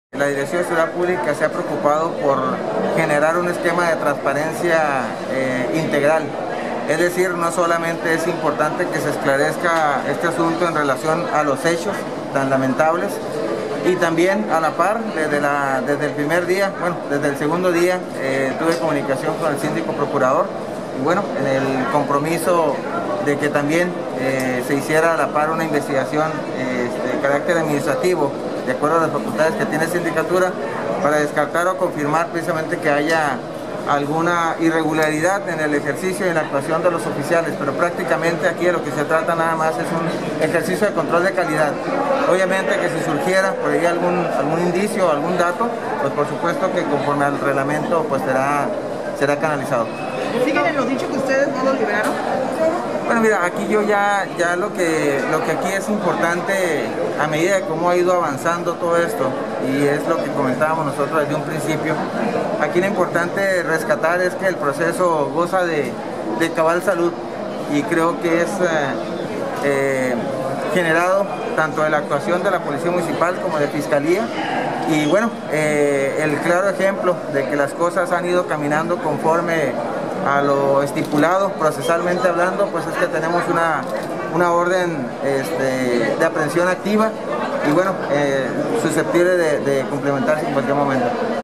PROCESO CONTRA PERSONA QUE ATROPELLÓ A MAMÁ E HIJA GOZA DE CABAL SALUD: DSPM
Que ya exista una orden de aprehensión contra el presunto responsable del atropellamiento y muerte de una mamá y su hija, en hechos suscitados el pasado 29 de mayo; significa que el proceso va por buen camino, considera el Director de la Policía Municipal de Mexicali